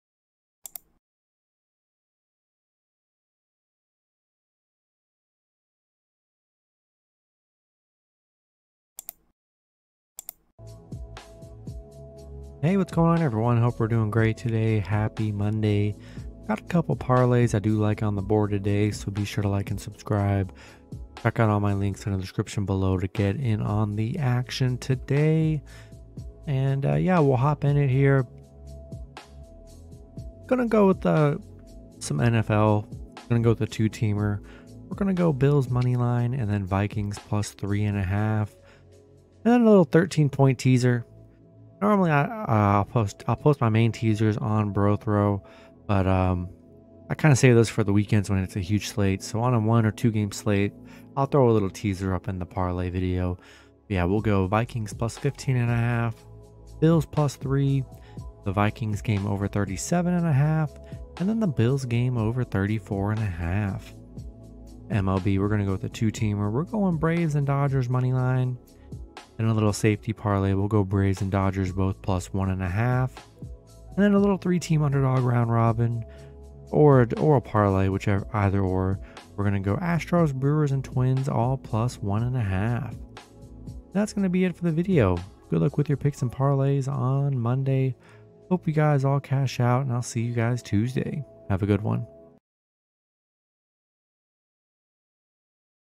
Free Multi-Sport Parlay Options for today 9/19/22
Here are my Free Multi sport parlay options for today

🏆Top Sportsbooks I use:
2. BOVADA
3. My Bookie

Music I use in my videos:
1. Music By The Relaxed Movement

✔️Parlays for today:
Bills ML
Vikings +3.5

13 Point Teaser
Vikings +15.5
Bills +3
Vikings Over 37.5
Bills Over 34.5

MLB
Braves ML
Dodgers ML

Braves +1.5
Dodgers +1.5

Astros +1.5
Brewers +1.5
Twins +1.5